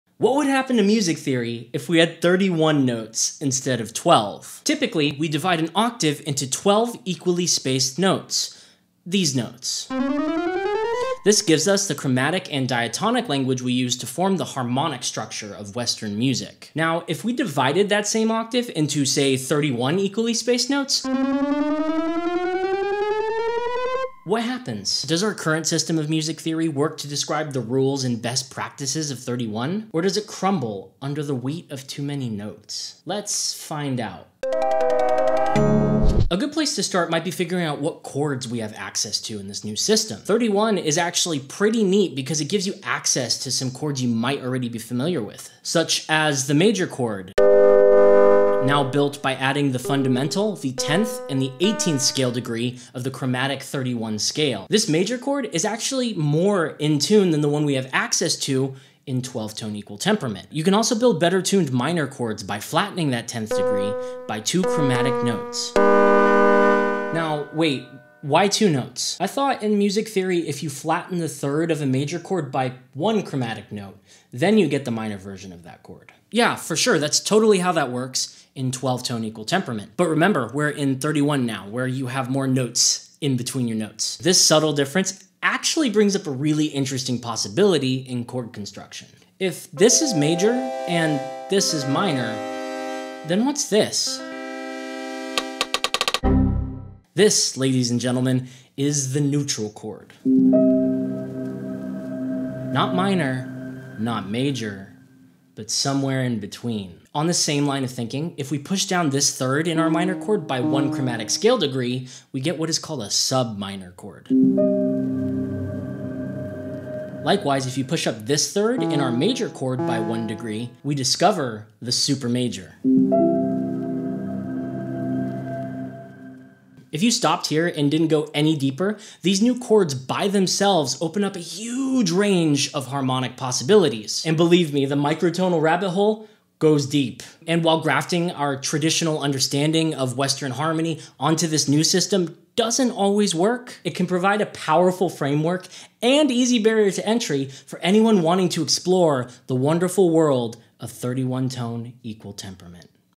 What If We Had 31 Notes Instead of 12?
How does music theory change if we had 31 notes instead of 12? Intro into 31EDO music theory! Ever wonder what kind of awesome chords and harmony we are missing out on by sticking to the rigid standards of 12 tone equal temperament? 12TET has had its time in the sun, now its time for it to step aside for 31TET! Join me in todays exploration of 31 tone equal temperament. Today I go over how to construct new chords like the neutral chord, the Subminor chord, and the supermajor chord! These microtonal chords gives us so much freedom when it comes to building unique harmony, microtonal chord progressions, and unique xenharmonic music.  I hope this microtonal music lesson gives you the tools to allow you to easily start making microtonal and xenharmonic music for yourself.

Follow the link to access my free sample pack

- Awesome microtonal resource for 31 notes, includes a standalone 31TET keyboard, list of intervals in 31 equal temperament, and notes on enharmonic equivelants, ambiguous harmony and how to do notation with 31 EDO

-Xenharmonic Wiki on 31 including interesting bits of music theory on the Neutral circle-of-fifths, Sagittal notation, and 31TET scales.